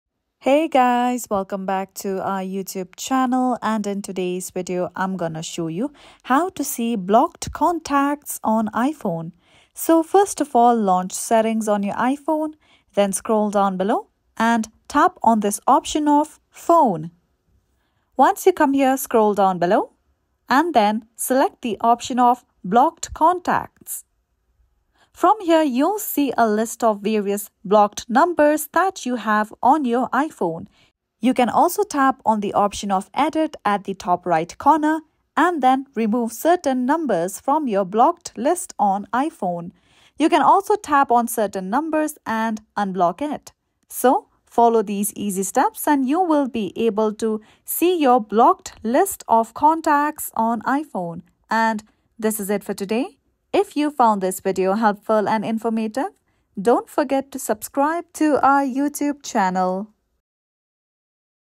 How to See Blocked Contacts on iPhone
In today's video, I'm going to show you how to see blocked contacts on your iPhone. It's a simple process that allows you to manage your blocked numbers easily.

Step-by-Step Guide:

Open Settings:

Launch Settings: Open the Settings app on your iPhone.
Access Phone Settings:

Scroll Down: Scroll down and tap on the option labeled "Phone."
View Blocked Contacts:

Blocked Contacts: Scroll down again and select "Blocked Contacts."
See the List: Here, you'll see a list of all the numbers you've blocked on your iPhone.
Manage Blocked Contacts:

Edit Blocked List: Tap on "Edit" at the top right corner to manage your blocked contacts.
Remove Contacts: You can remove certain numbers from your blocked list by tapping the red minus icon next to the contact and then tapping "Unblock."
By following these easy steps, you can view and manage your blocked contacts on your iPhone. It's a great way to keep your blocked list up to date and ensure you're not missing any important calls or messages.